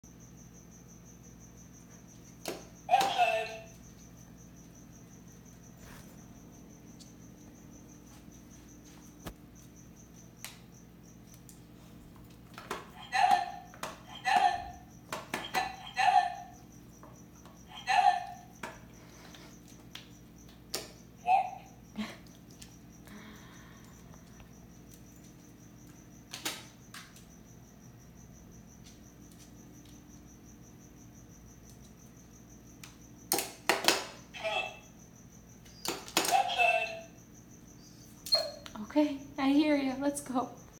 Stella Combining Words!
To learn more about how I taught Stella to talk and how you can get started with your dog at home, you can pre-order my book

Giving Stella increased wait time while she's talking allows her to combine words and make more complex messages on her AAC device!